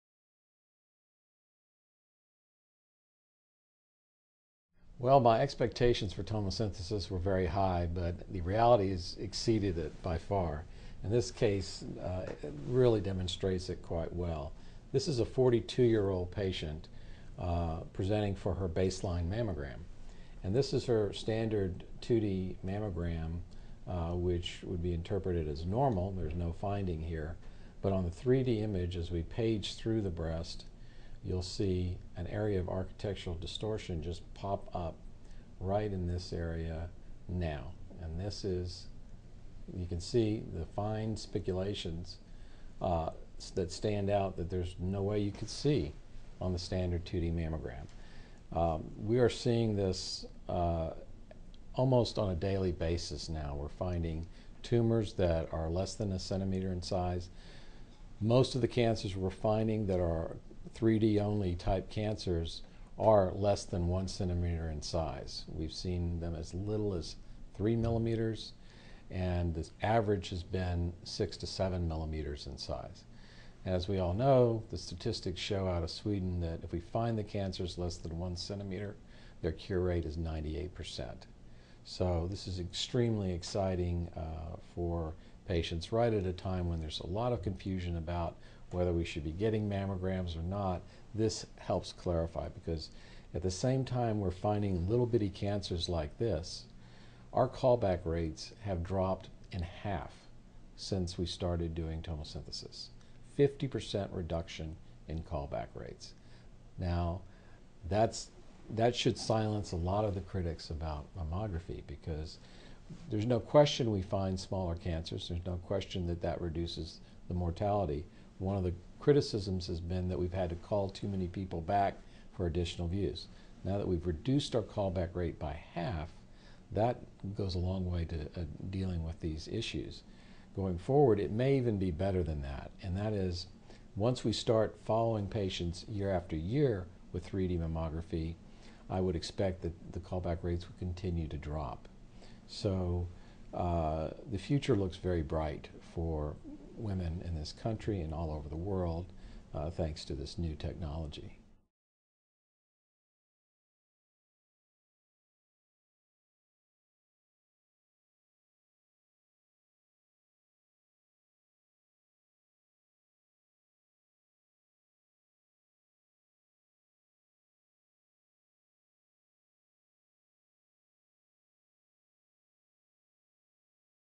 Breast Cancer Screening with 3D Mammography-- Dr. Rose
Hear Dr. Rose explain how he used 3D mammography to find breast cancer in a 42-year-old woman, and watch him demonstrate how there was no way to see this cancer in a standard mammogram. Dr. Rose says he believes 3D mammography is extremely exciting for women because this breast cancer screening tool is finding cancer early, when it's smaller and easiest to cure.

During the Hologic 3D mammography procedure, the x-ray arm sweeps in as light arc over the breast, taking multiple images in just seconds. The Hologic 3D mammography system uses high-powered computing to convert the images into a 3D image, which can then be examined by your doctor one page or "slice" at a time.

Instead of viewing all the complexities of your breast tissue superimposed in a flat image as with a traditional mammogram, the Hologic 3D mammography system allows the doctor to examine the breast tissue layer by layer. Fine details are more clearly visible, no longer hidden by the tissue above and below.

Using the Hologic 3D mammography system for screening has been proven to reduce "call-backs" or false positives by 40 percent.1 In addition, the Hologic 3D mammography procedure has also been shown in clinical studies to be more accurate than conventional mammography alone by detecting cancers earlier.2

Learn more about breast cancer screening and find your 3D mammography provider

1 Philpotts, Liane, et. al. "Initial Experience With Digital Breast Tomosynthesis in Screening Mammography." Paper presented at the annual meeting of the American Roentgen Ray Society Annual Meeting, Vancouver, British Columbia, May 3, 2012.
2 Skaane, Per, et. al. "Reading Time of FFDM and Tomosynthesis in a Population-based Screening Program." Paper presented at the annual meeting of the Radiological Society of North America, Chicago, Illinois, November 29, 2011.